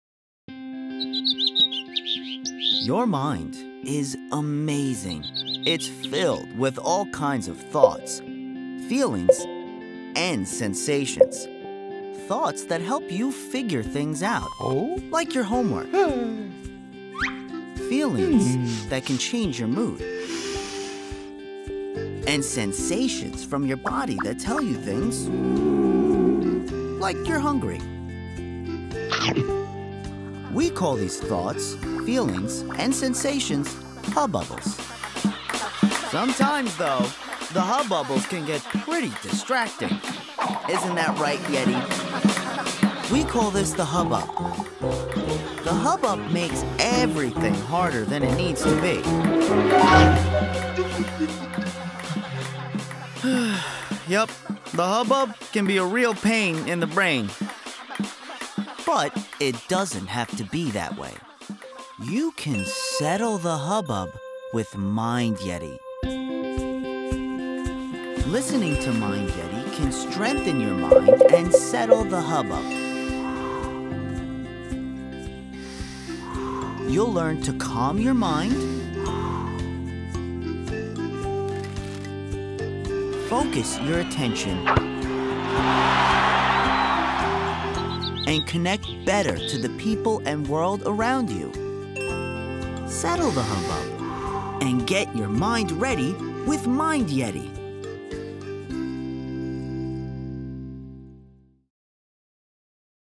Get Your Mind Ready with Mind Yeti
Mind Yeti
Mind Yeti is a research-based digital library designed to help kids and their adults calm their minds, focus their attention, and connect better to the world around them.